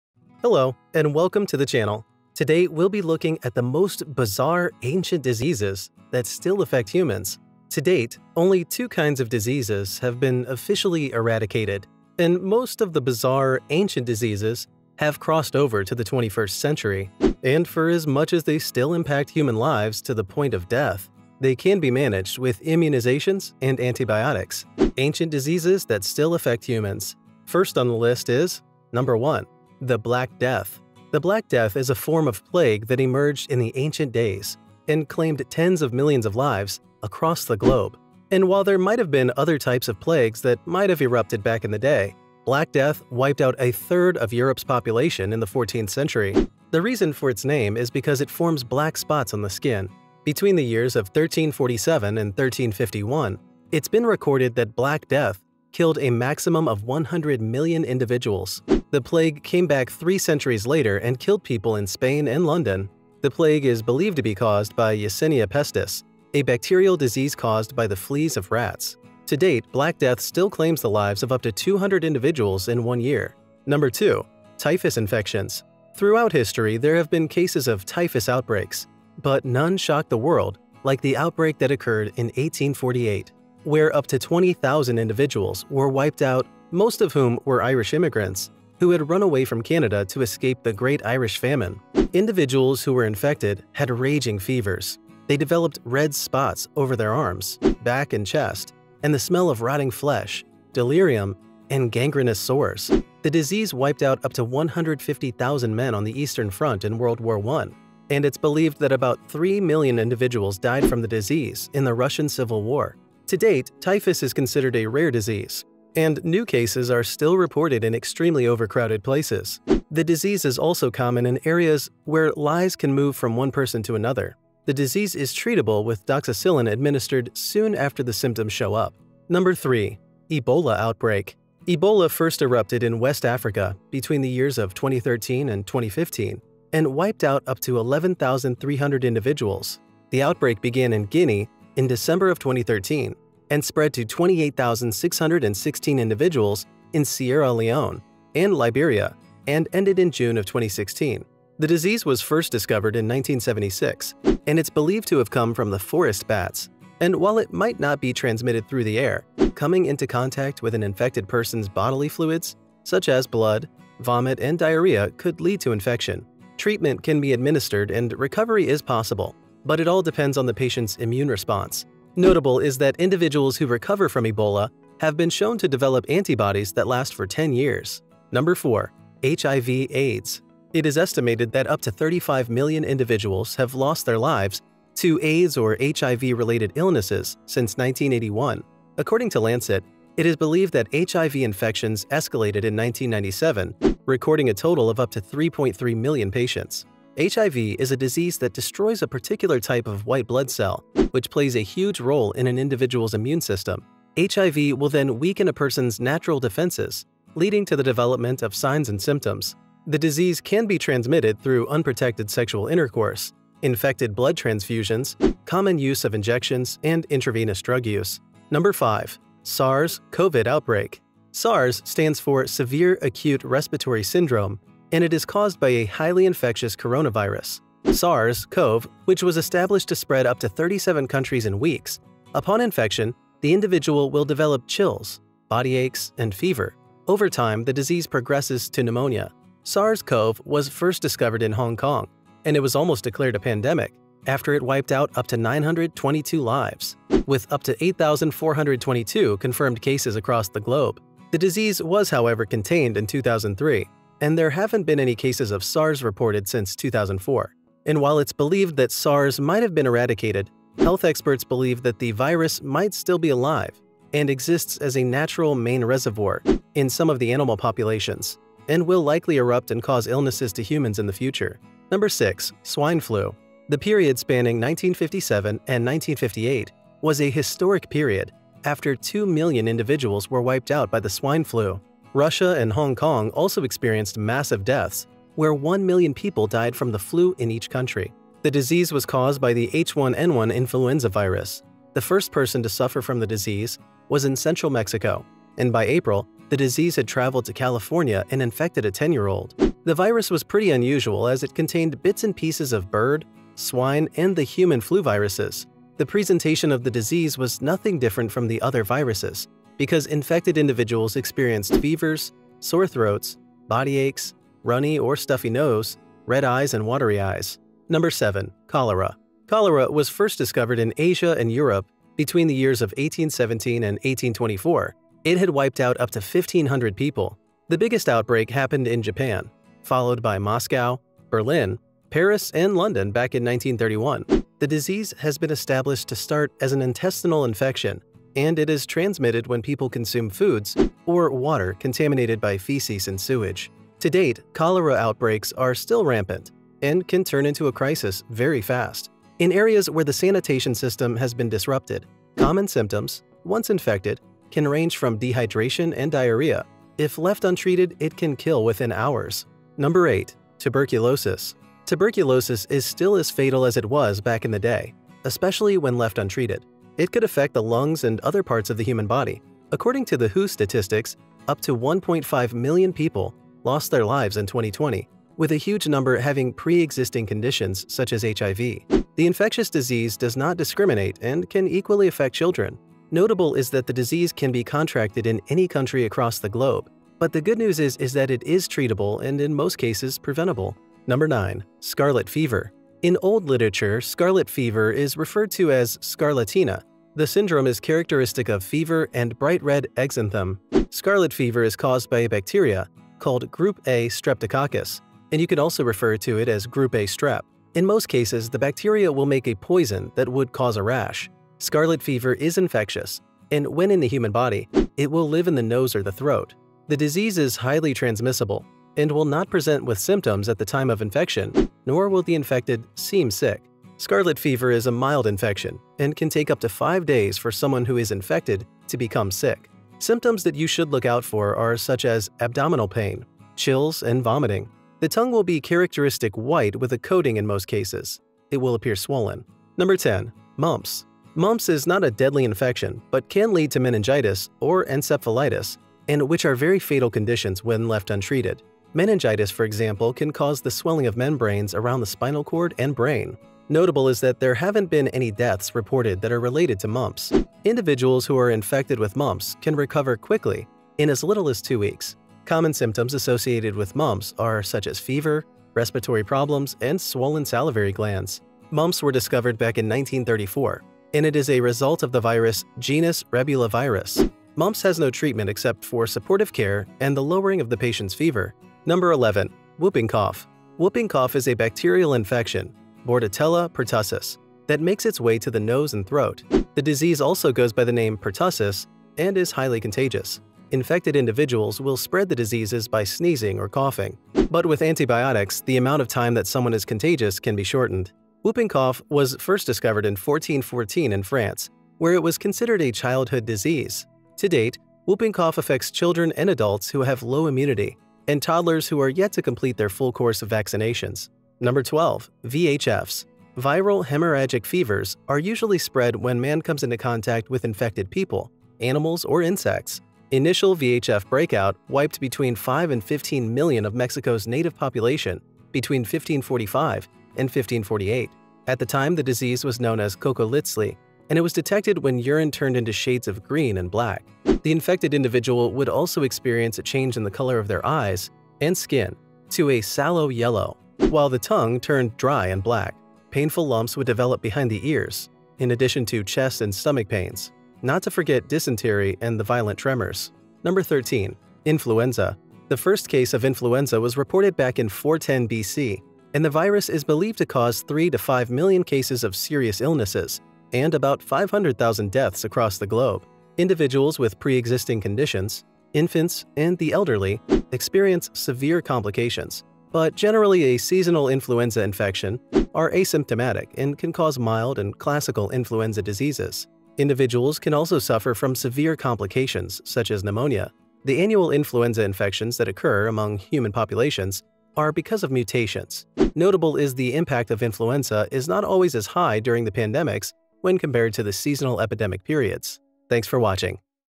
13 Most Bizarre Ancient Diseases That Still Affect Humans
13 Most Bizarre Ancient Diseases That Still Affect Humans. Today we will be looking at the Most Bizzare Ancient Diseases That Still Affect Humans.

To date only two kinds of diseases have been officially eradicated, and most of the bizarre ancient diseases have crossed over to the 21st century. And for as much as they still impact human lives to the point of death, they can be managed with immunizations and antibiotics.

Ancient Diseases that Still Affect Humans

1. The Black Death
Is a form of plague that emerged in the ancient days and claimed tens of millions of lives across the globe. And while there might have been other types of plagues that might have erupted back in the day, Black Death wiped out a third of Europe’s population in the 14th century. The reason for its name is because it forms black spots on the skin.

2. Typhus Infections
Throughout history, there have been cases of typhus outbreaks but none shocked the world like the outbreak that occurred in 1848. Where up to 20,000 individuals were wiped out most of whom were Irish immigrants who had run away from Canada to escape the great Irish famine. Individuals who were infected had raging fevers, they developed red sports over the arms, back, and chest, the smell of rotting flesh, delirium, and gangrenous sores.

3. Ebola Outbreak
Ebola first erupted in West Africa between the years of 2013 and 2015 and wiped out up to 11,300 individuals. The outbreak began in Guinea in December of 2013 and spread to 28,616 individuals in Sierra Leone and Liberia and ended in June of 2016. The disease was first discovered in 1976 and it is believed to have come from the forest bats. And while it might not be transmitted through air, coming into contact with an infected person's bodily fluids such as blood, vomit, and diarrhea could lead to infection.